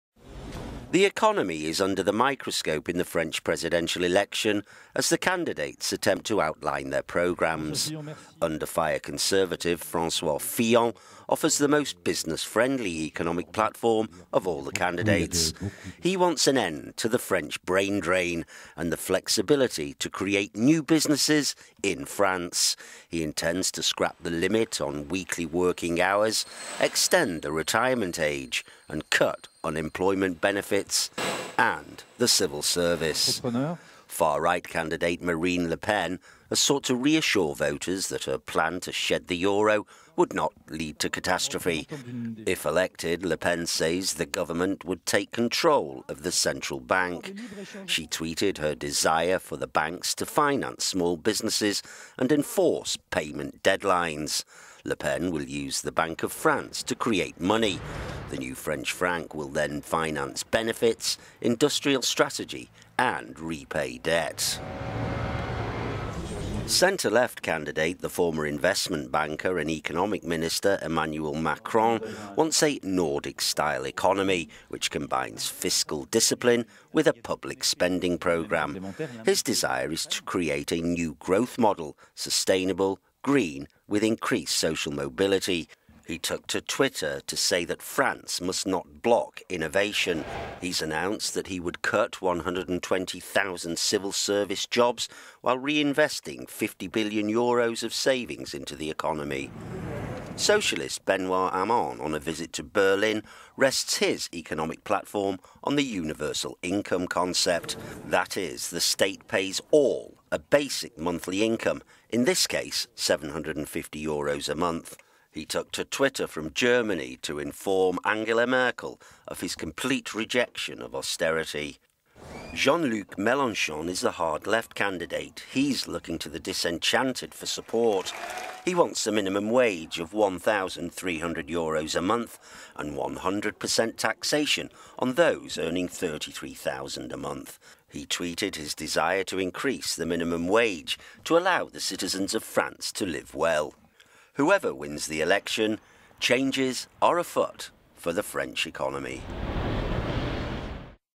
French presidential candidates outline economic intentions - economy
The economy came under the microscope in the French presidential election as the candidates outlined their programmes.

Under fire Conservative François Fillon offers the most business-friendly economic platform of all the candidates.

France's justice minister dismisses wiretap accusations by Francois Fillon Bloomberg (@business) March 27, 2017

He wants an end to the French brain drain and demands flexibility to create new businesses in …